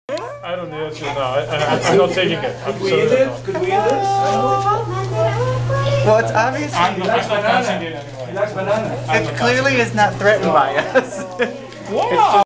Couchia (acouchi) eating a banana
This little critter was cute and fun to watch.  I think in English they're called acouchi, but it Portuguese it's a couchia (spelling?!).  Anyhow, Father Ugge told un we better enjoy watching it because it would probably be dinner in the near future!